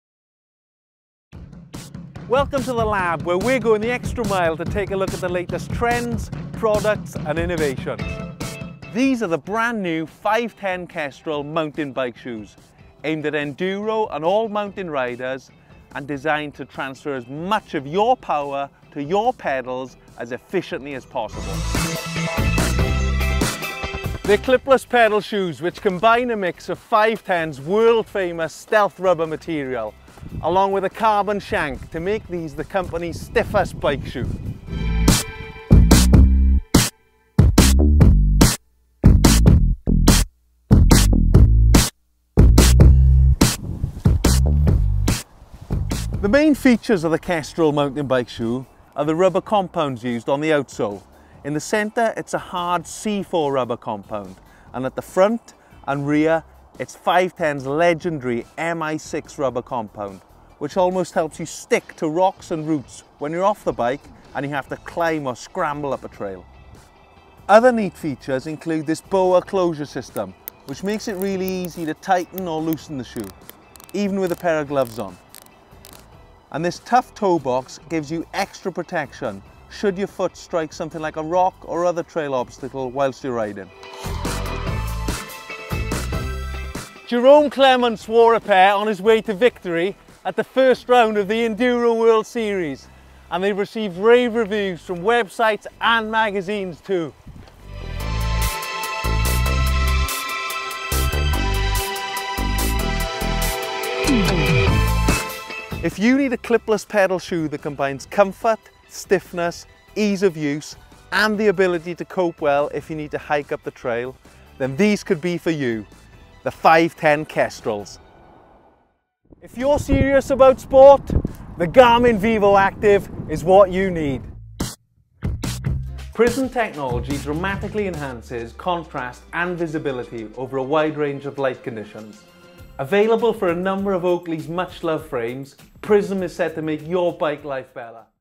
FiveTen Kestrel review
The new 5.10 Kestrel is designed to transfer power to the pedals as efficiently as possible using a low-profile design and snug Boa closures to transform human power into horse power.

Some may think that the Kestrels are solely intended for experiencing the love of riding a mountain bike, and others believe they were designed for enduro. Dont be fooled-- whatever your passion, the Kestrels are intended to get you out and pedalling.